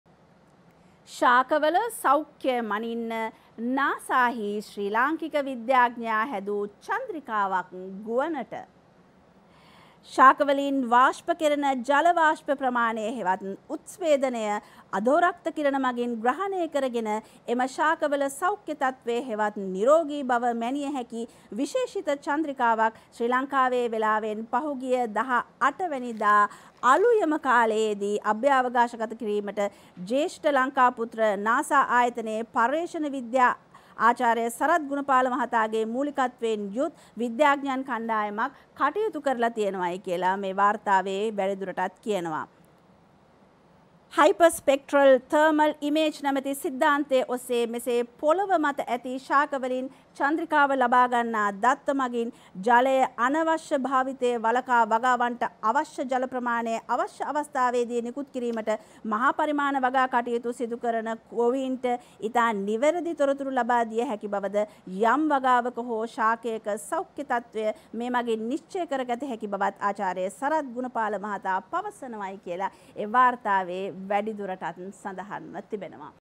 ශාකවල සෞඛ්‍යය මනින්නනාසා හි ශ්‍රී ලාංකික විද්‍යාඥයා හැදූ චන්ද්‍රිකාවක් ගුවනට
Live at 12 සහ Live at 08 සමබර ප්‍රවෘත්ති ප්‍රකාශය